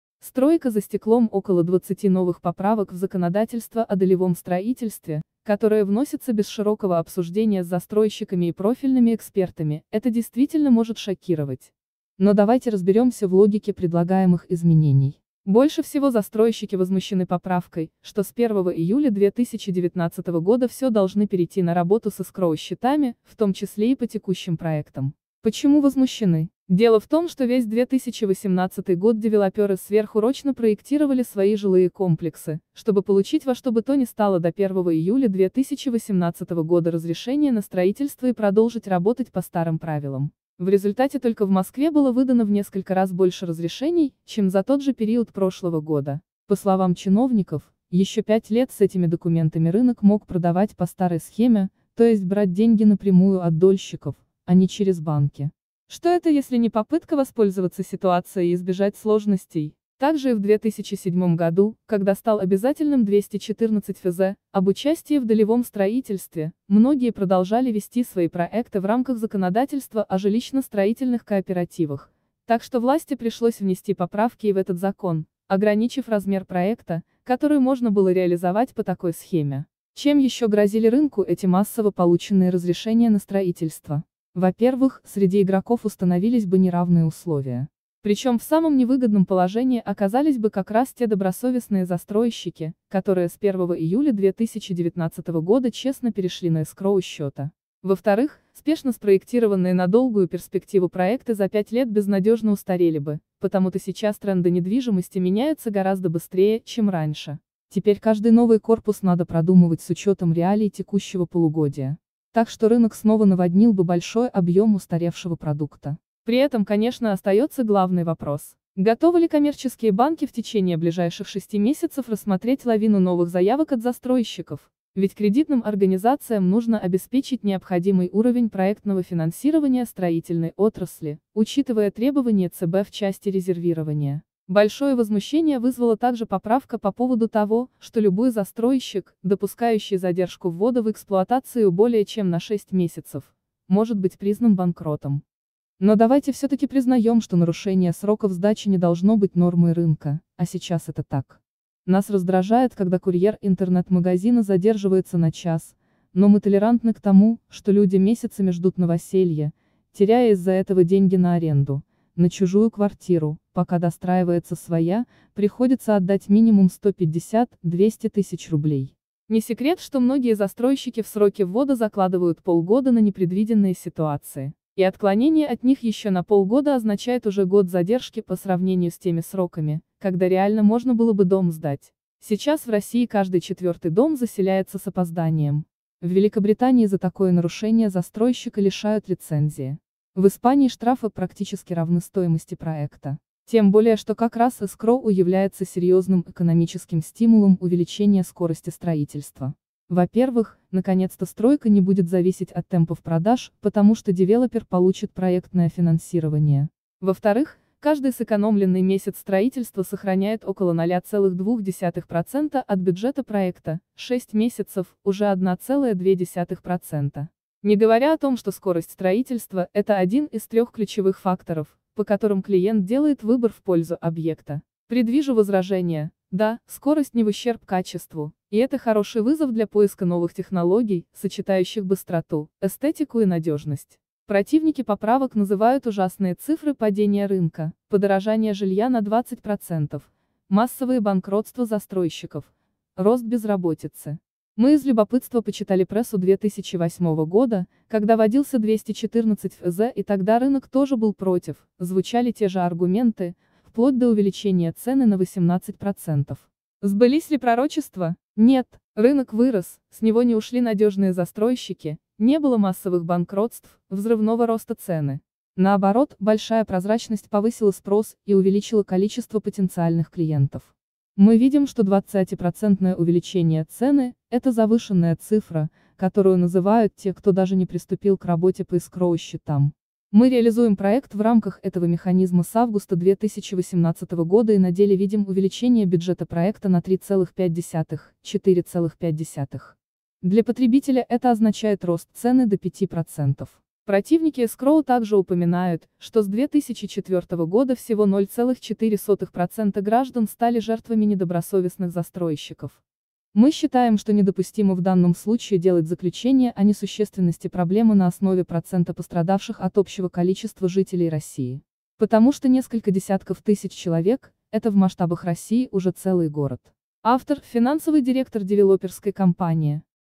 Стройка за стеклом
Стройка за стеклом Около 20 новых поправок в законодательство о долевом строительстве, которые вносятся без широкого обсуждения с застройщиками и профильными экспертами, это действительно может шокировать . Но давайте разберемся в логике предлагаемых изменений . Больше всего застройщики возмущены поправкой, что с 1 июля 2019 года все должны перейти на работу с эскроу-счетами, в том числе и по текущим проектам . Почему возмущены Дело в том, что весь 2018 год девелоперы сверхурочно проектировали свои жилые